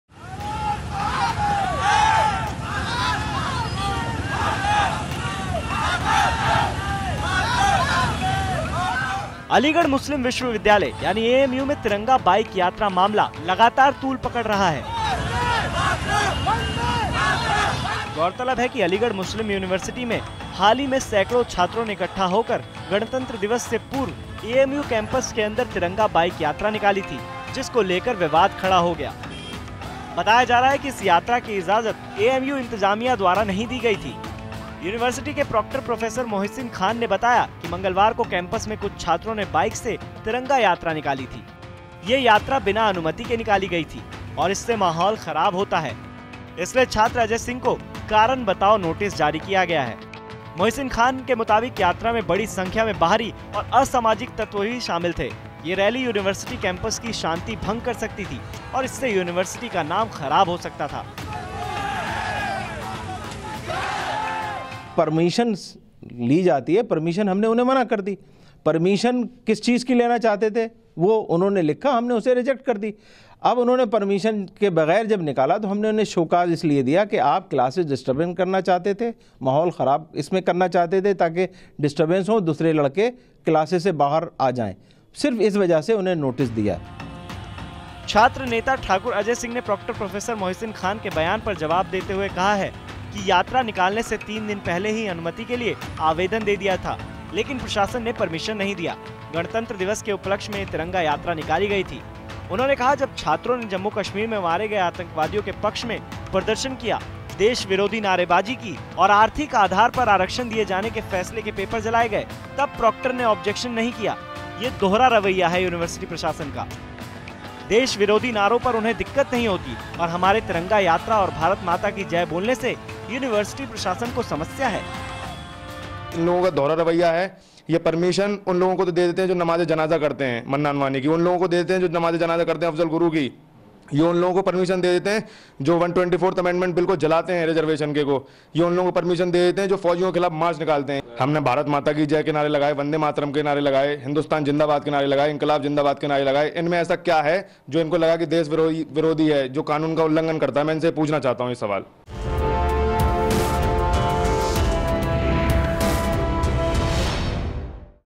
AMU में तिरंगा यात्रा विवाद बढ़ा, दो हिंदू छात्र नेताओं को भेजा नोटिस! | UP Tak
उत्‍तर प्रदेश के अलीगढ़ मुस्लिम विश्वविद्यालय (एएमयू) में तिरंगा बाइक यात्रा मामला लगातार तूल पकड़ रहा है...